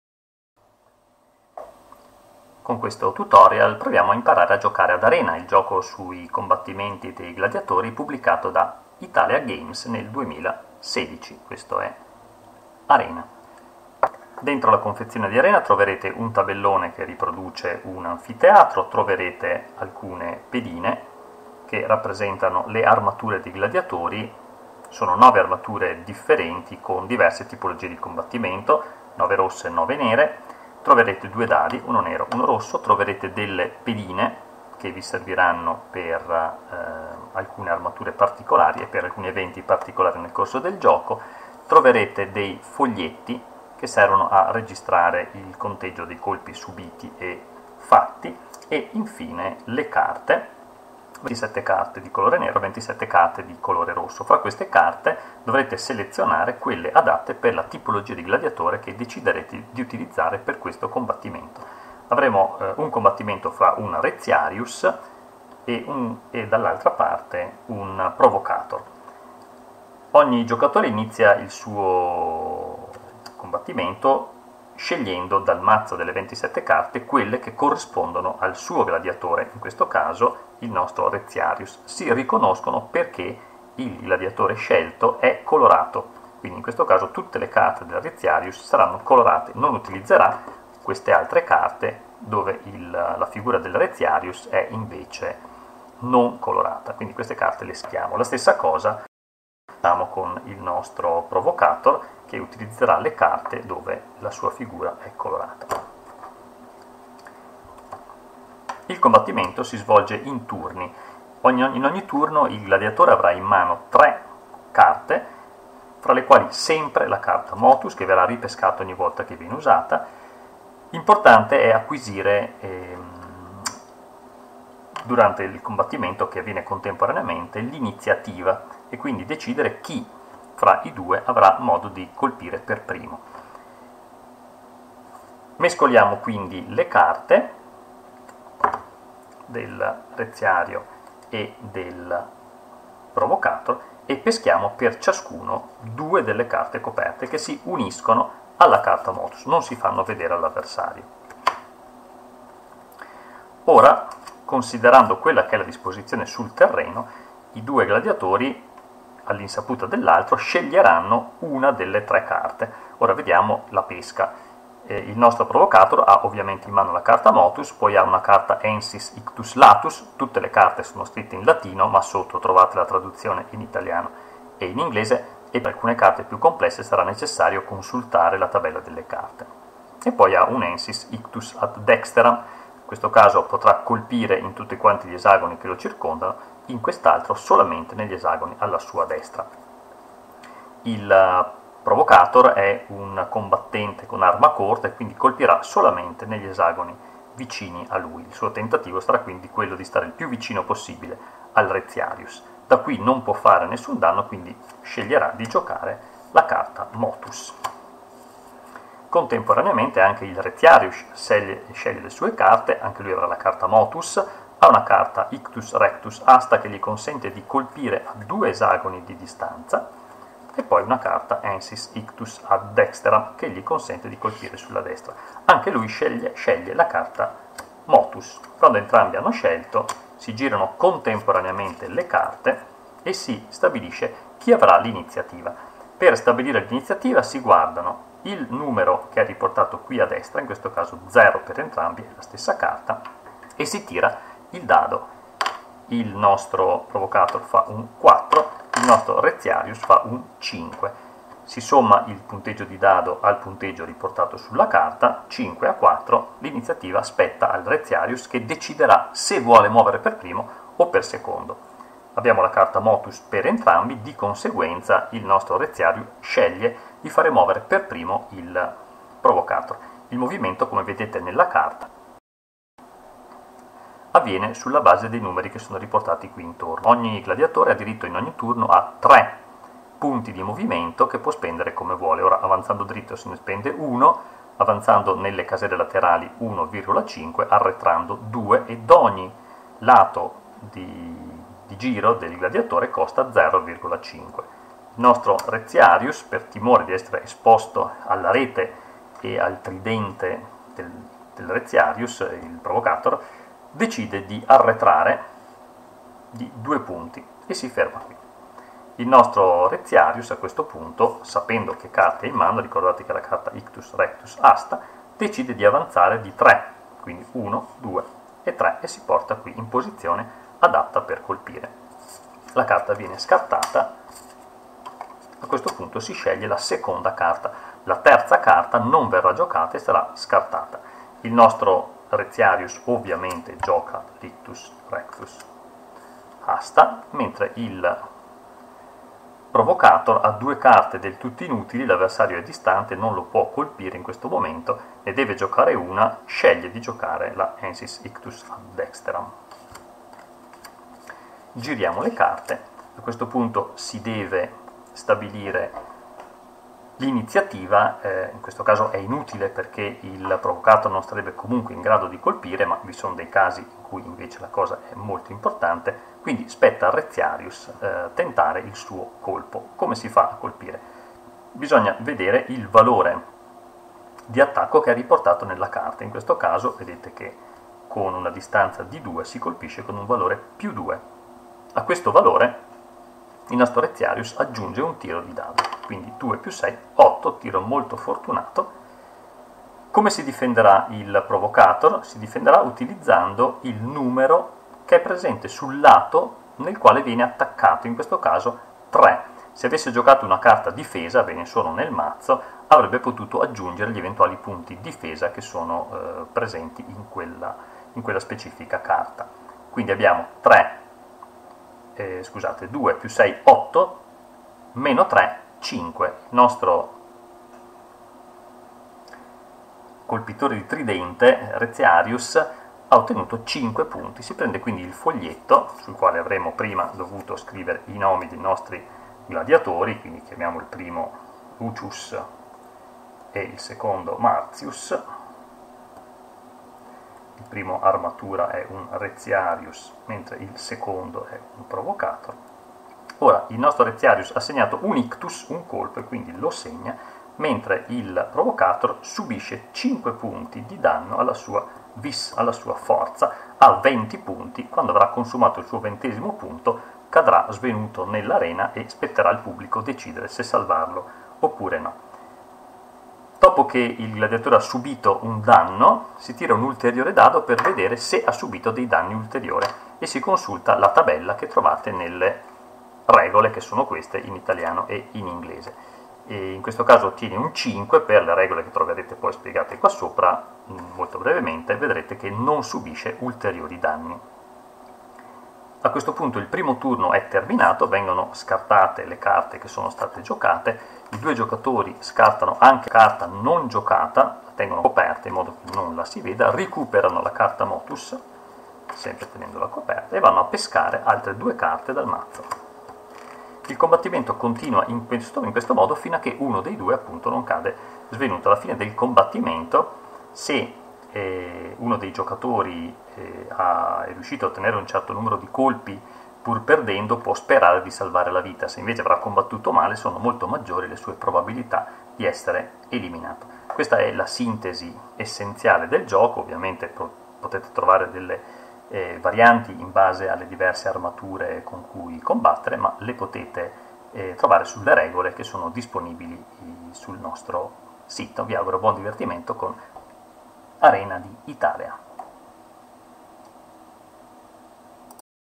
tutorial Arena 1
Tutorial per imparare a giocare ad Arena - ItAlea Games.